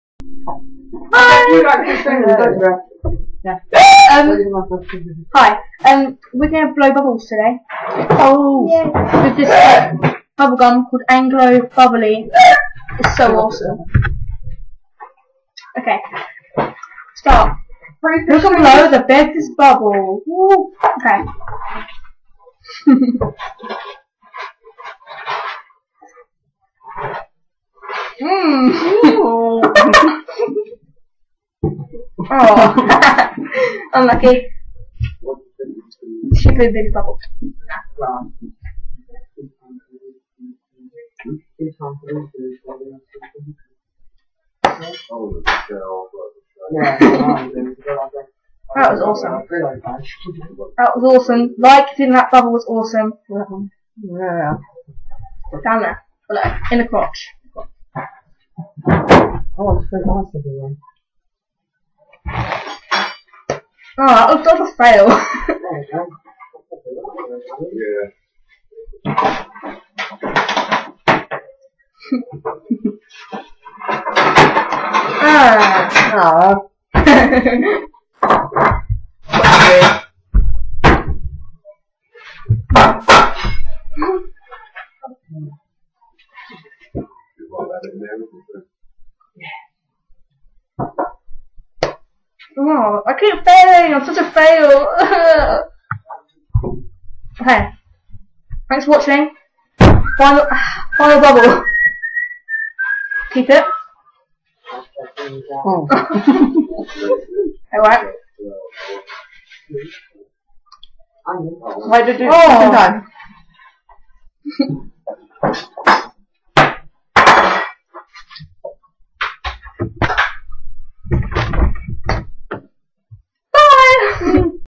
Sexy bubble blowing!!
I fail at blowing bubbles! lol. Hope you like da sexy bubbles which fail! lol. :D sub and like 4 more stuff! :) xxx also. we are starting this question and answer video thing so feel free to inbox me or amazbabe a question and we will answer it! Make sure that you address it to either millie (me) or beth (amazbabe) THANK YOU!!!